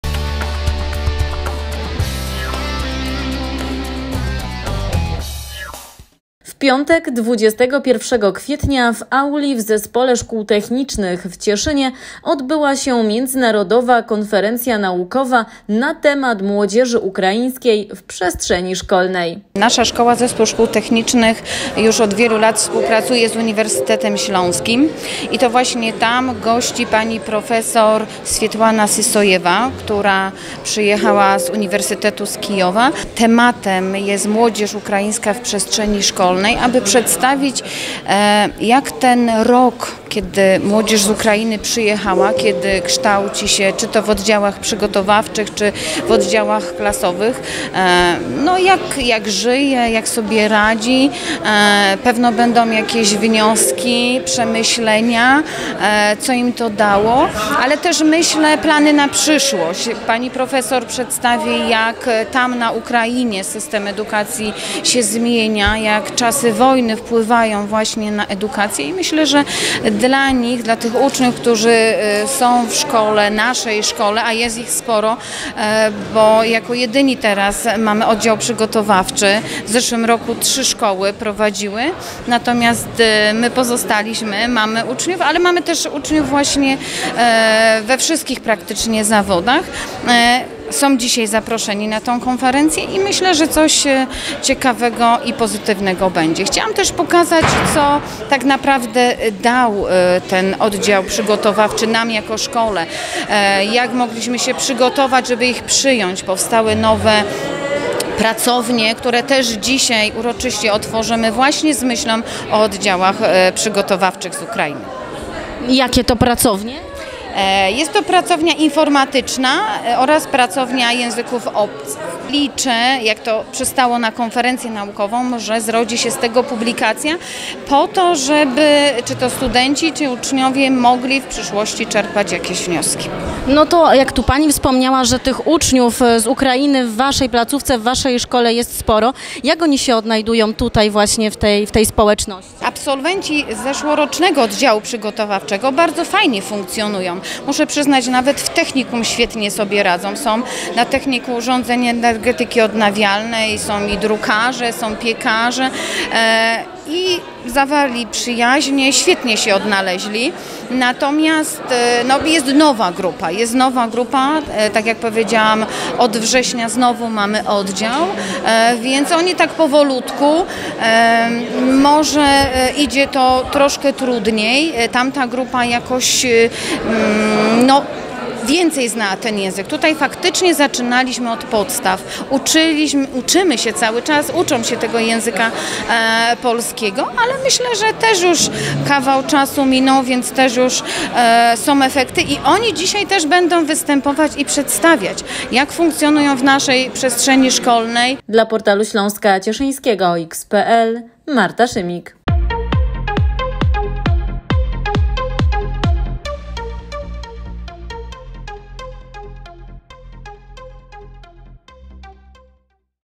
"Młodzież ukraińska w przestrzeni szkolnej"
W piątek (21.04) w Zespole Szkół Technicznych w Cieszynie odbyła się międzynarodowa konferencja naukowa "Młodzież ukraińska w przestrzeni szkolnej".